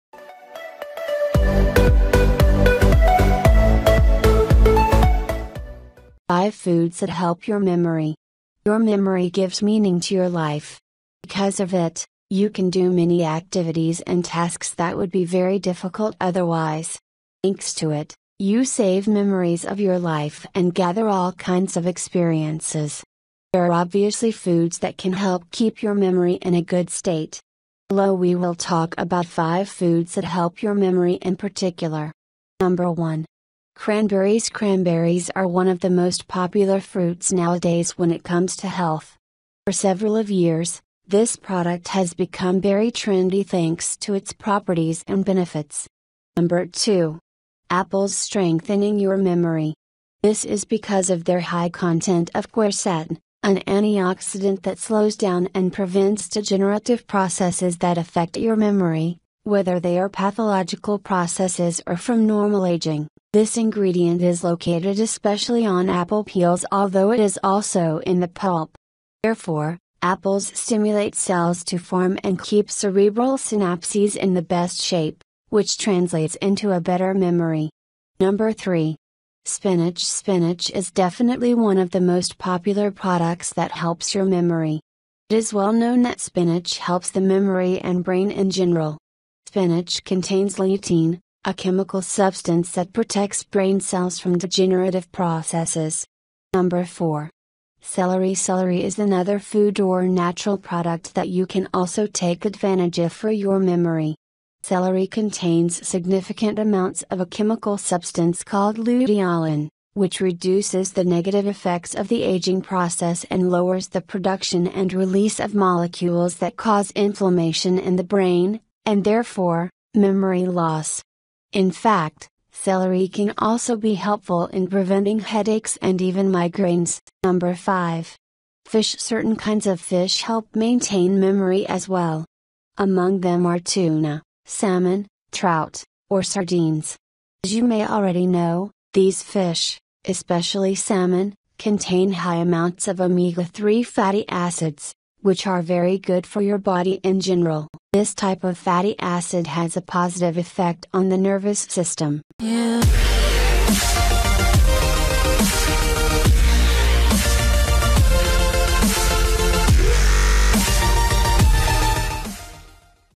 5 Foods that Help Your Memory | Health Cure & Tips
5 Foods that Help Your Memory

5 Foods that Help Your Memory

Your memory gives meaning to your life.

Because of it, you can do many activities and tasks that would be very difficult otherwise.

Thanks to it, you save memories of your life and gather all kinds of experiences.

There are obviously foods that can help keep your memory in a good state.

Below we will talk about 5 foods that help your memory in particular.

#1. Cranberries
#2. Apples
#3. Spinach
#4. Celery
#5. Fish